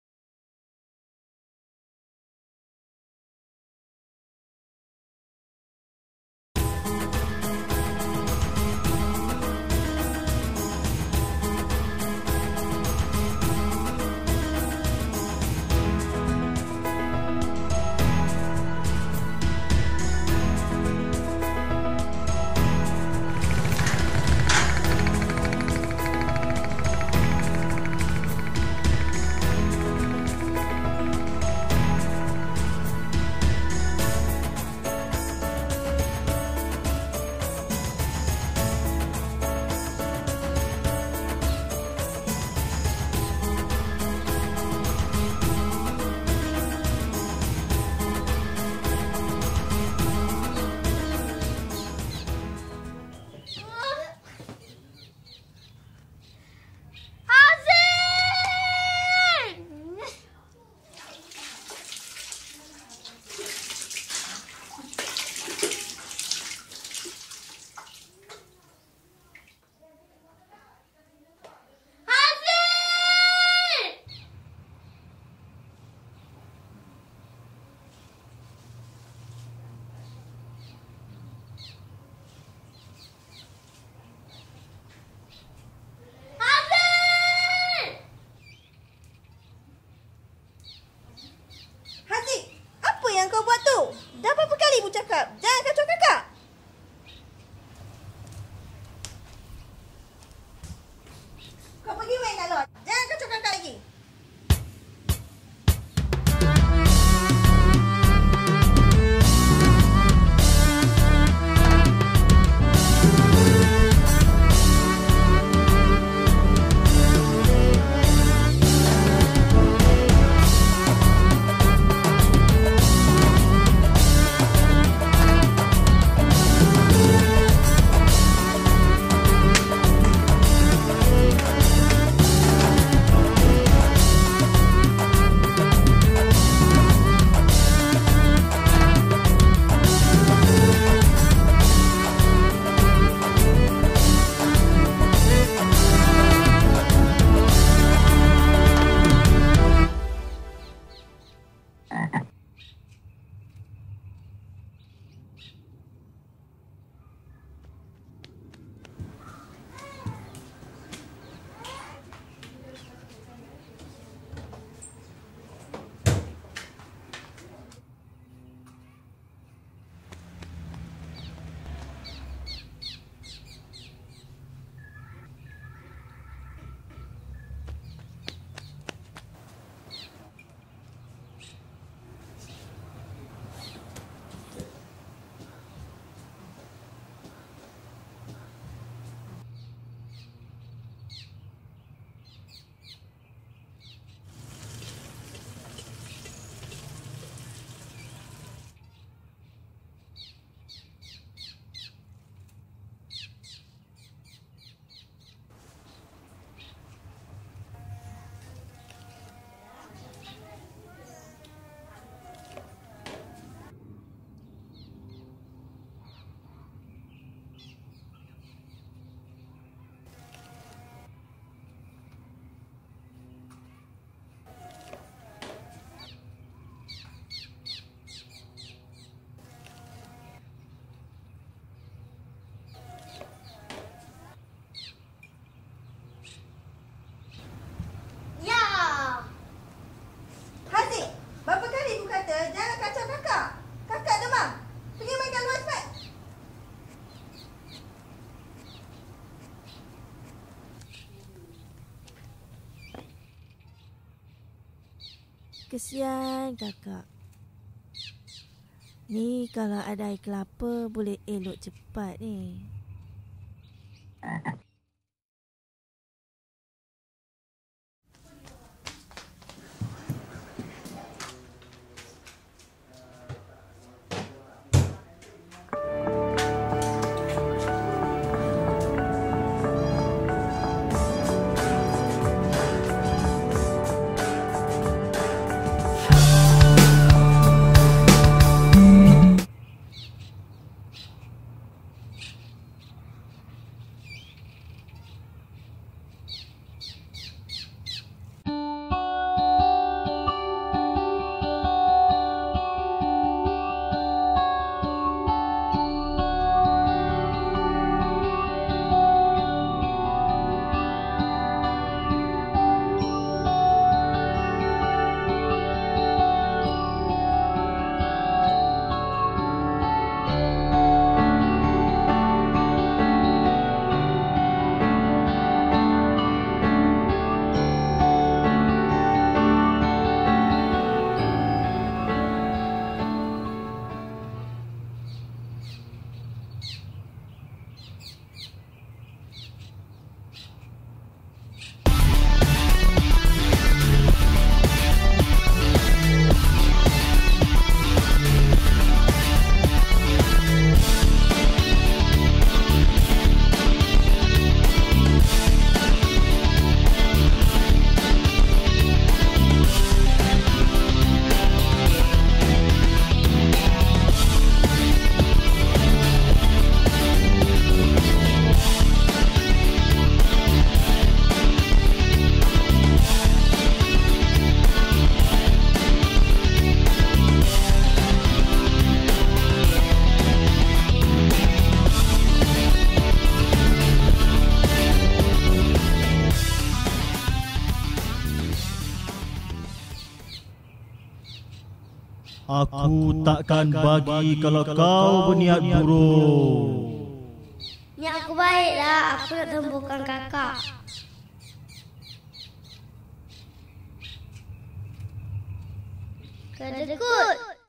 KELAPA pt. 1
This is a story of one naughty little boy's search for a remedy to help cure his sister's illness. In the process of searching, he discovers that life is not all about fun and games, and this story captures his journey in discovering his creativity, resilience and the meaning of growing up. Or does he?